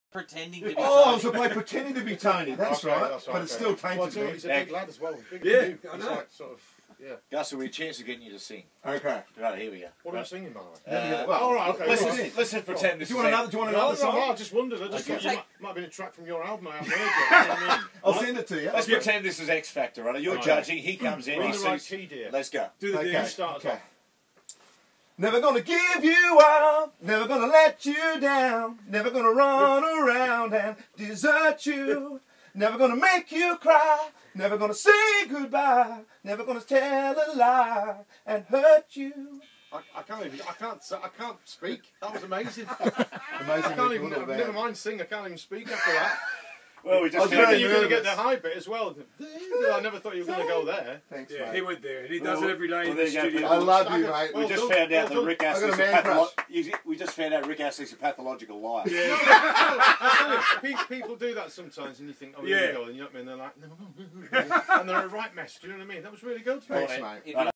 Rick Astley Watches Gus Worland Sing
Triple M's Gus Worland sings a Rick Astley song to Rick Astley!